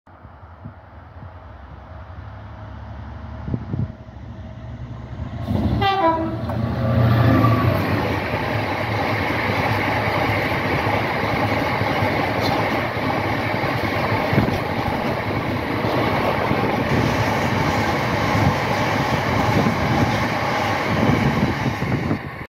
66729 passes through Chesterfield
GBRF locomotive 66729 'Derby County'  passes through Chesterfield Railway Station (Platform 1) on Wednesday September 4th 2024, hauling a rake of hoppers.